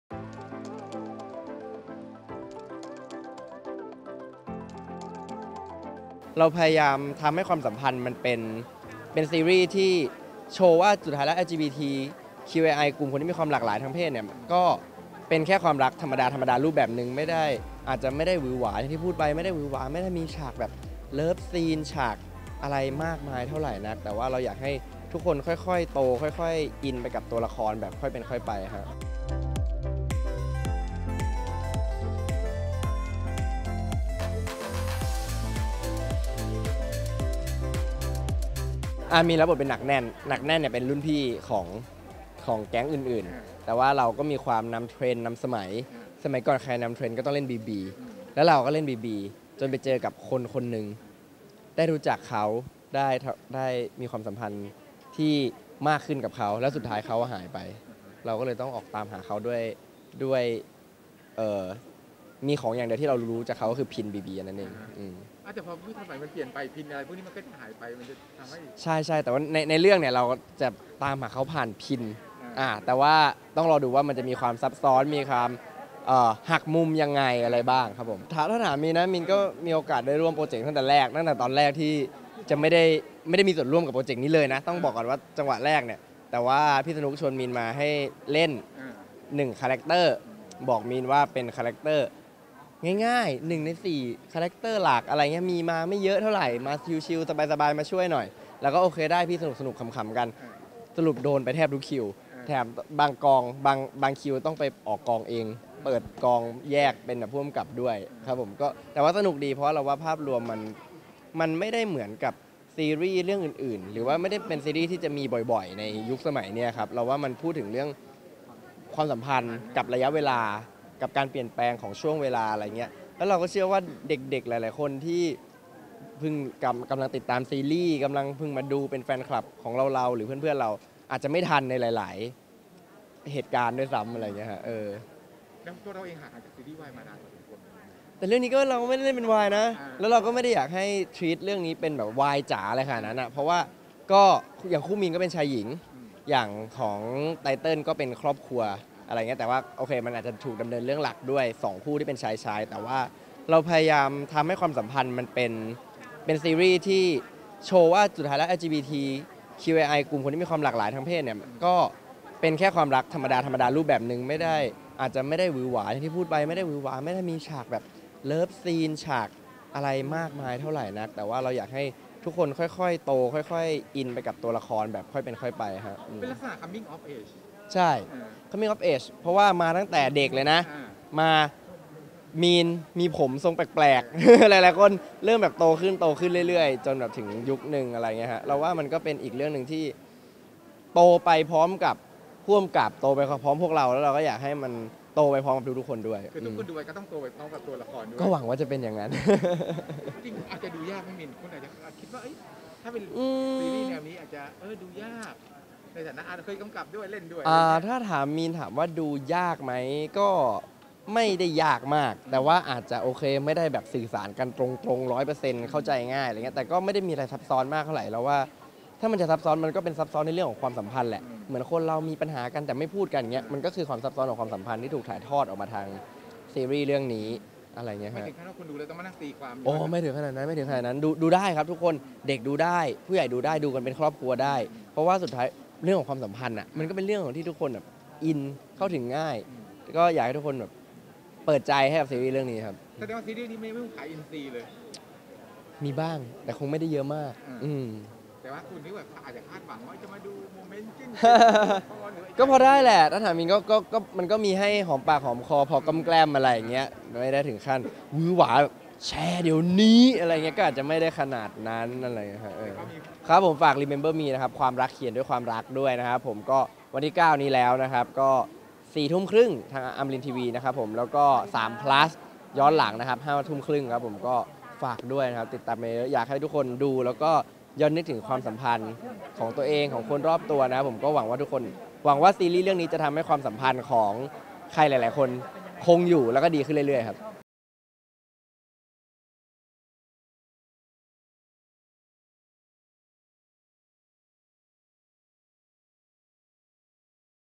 มีน พีรวิชญ์ เผยภาพรวมซีรีส์ Remember me
ภาพรวมมันไม่ได้เหมือนซีรีส์ เรื่องอื่นๆ  ไม่ได้เป็นซีรส์ที่จะมีบ่อยๆ ในยุคสมัยนี้ กับการเปลี่ยนแปลงของช่วงเวลา เชื่อว่าเด็กหลายคนในยุคนี้อาจจะไม่ทันกับช่วงเวลาดังกล่าว